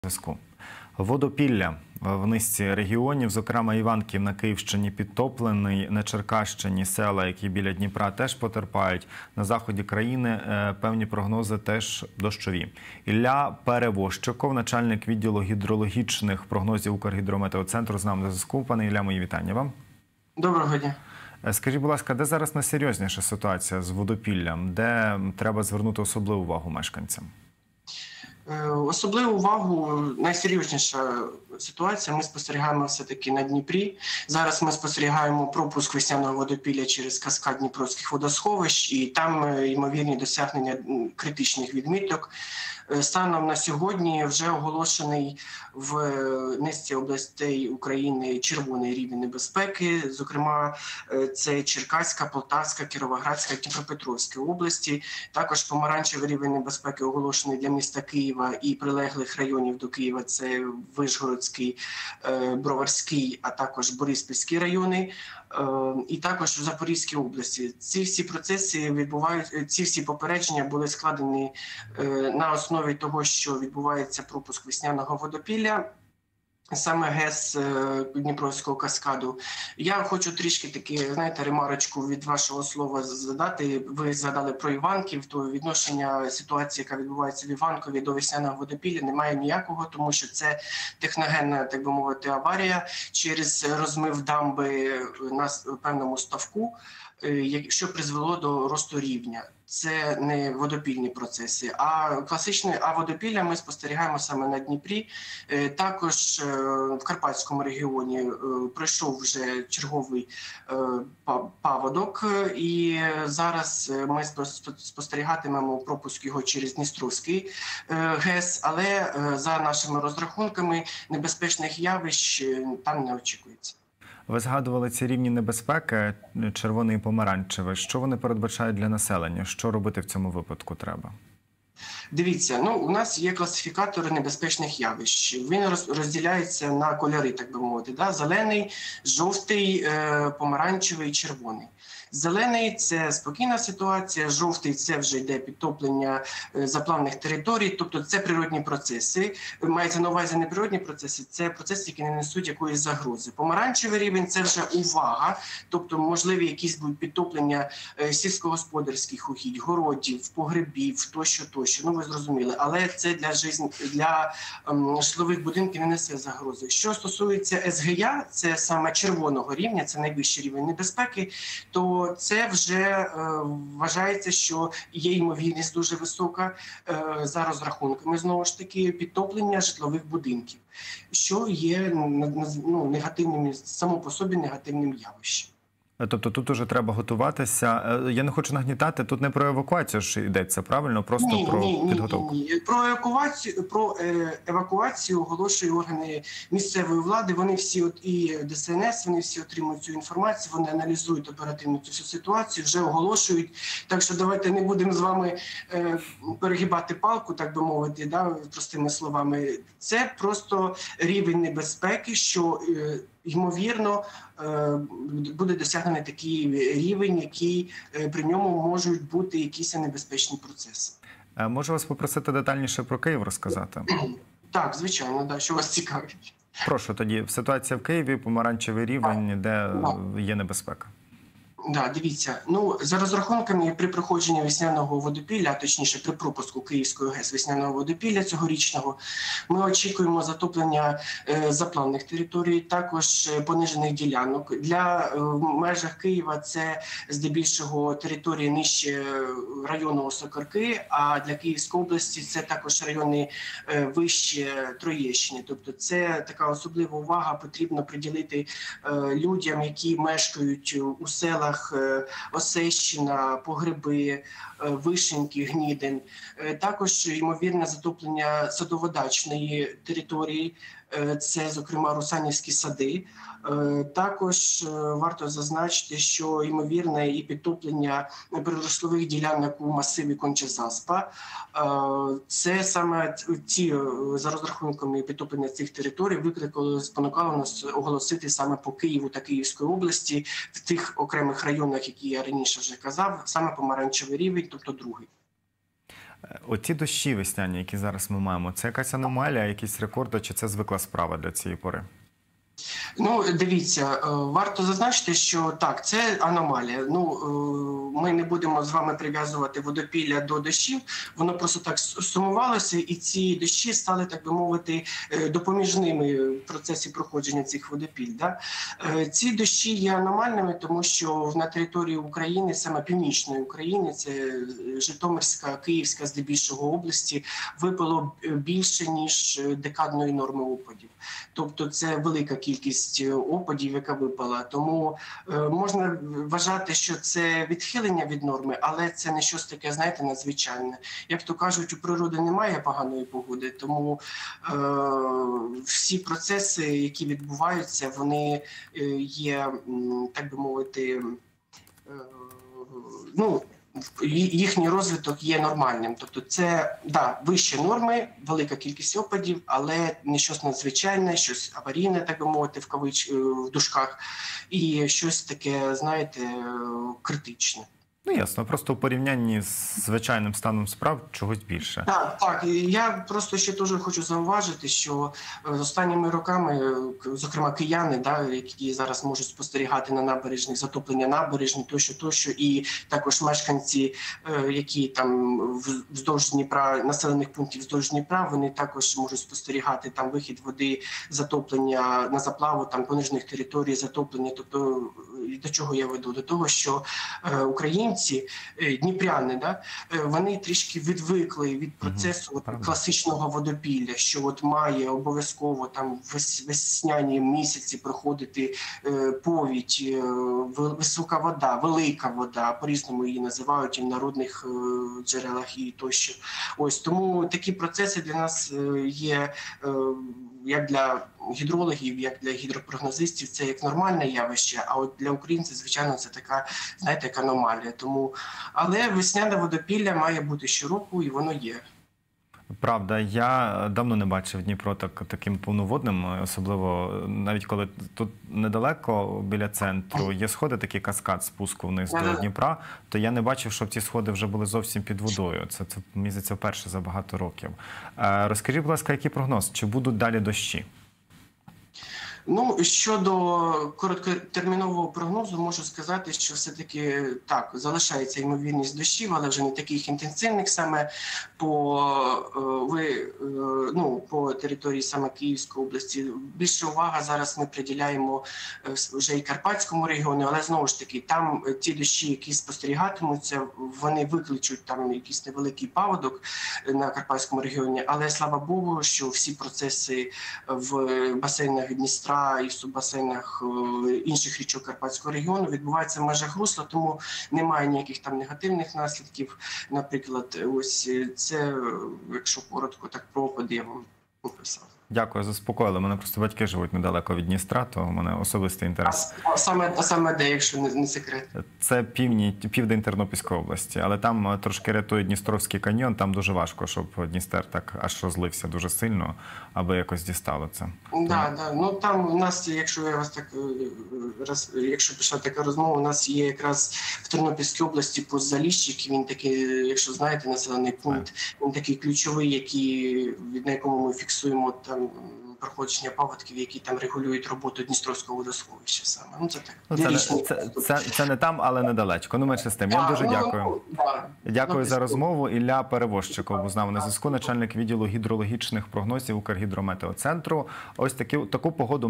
Весняне водопілля: кому готуватись до можливих підтоплень – прогнози "Укргідрометцентру"
Ілля Перевозчиков, начальник відділу гідрологічних прогнозів Укргідрометцентру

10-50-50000 - будь-яка сума має значення.

Разом переможемо!
Слава Україні!